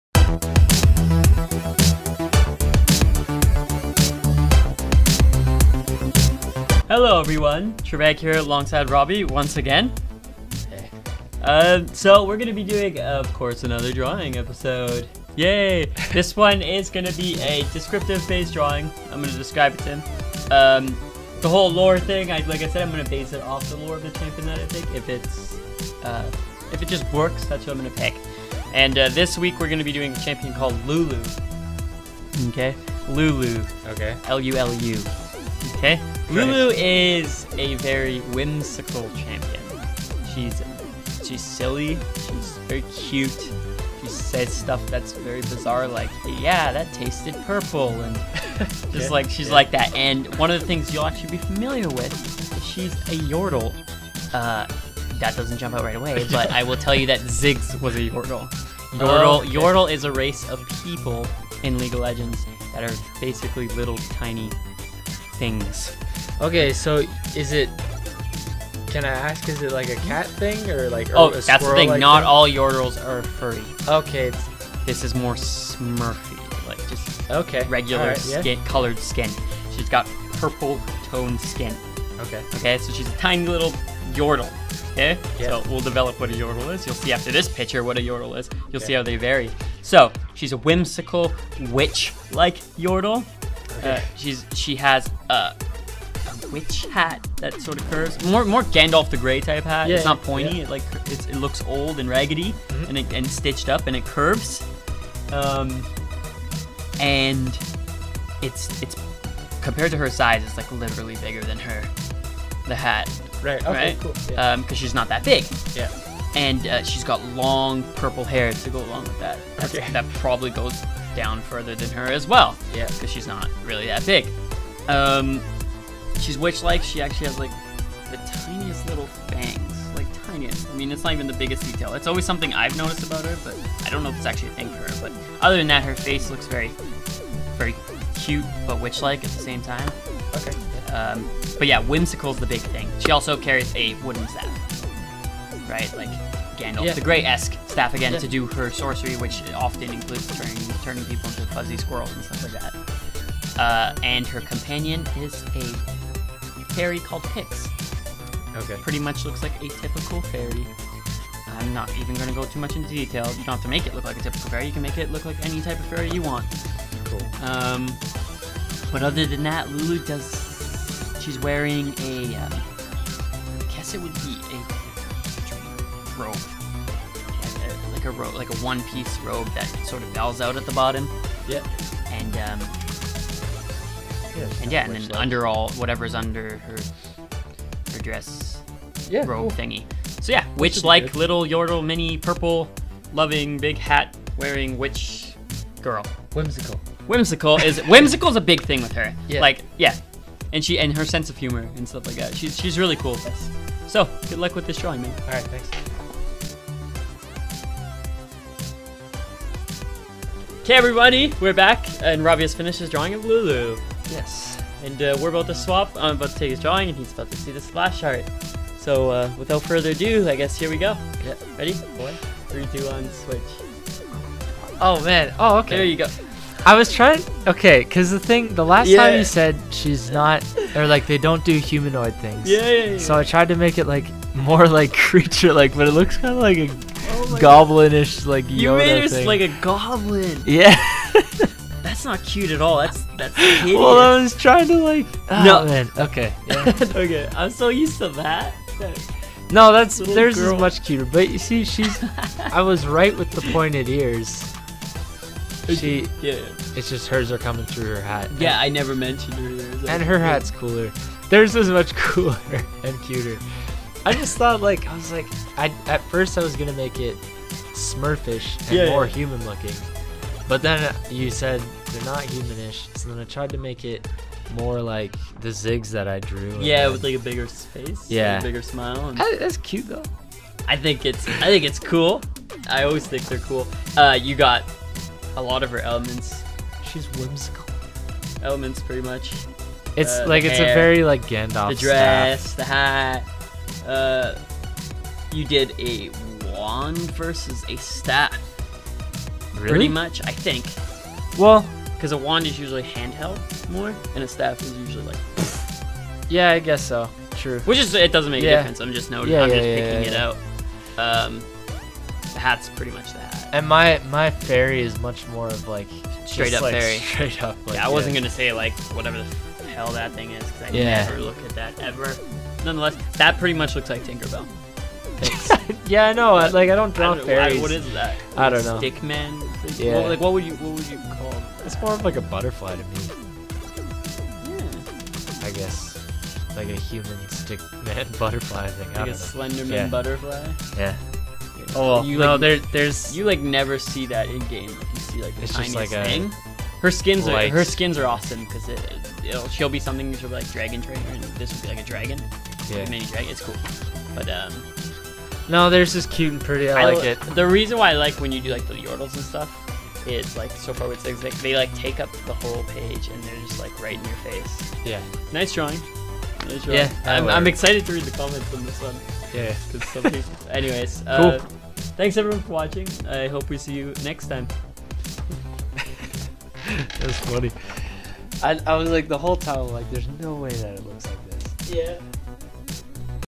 My Friend Drawing Champions He Has Never Seen - Ep 10 Lulu - League of Legends
Live-streamed drawing coming up in the next couple weeks (hopefully) Follow Also, expect more lore based episodes!
Pics will be uploaded to my Facebook page below. Who is next?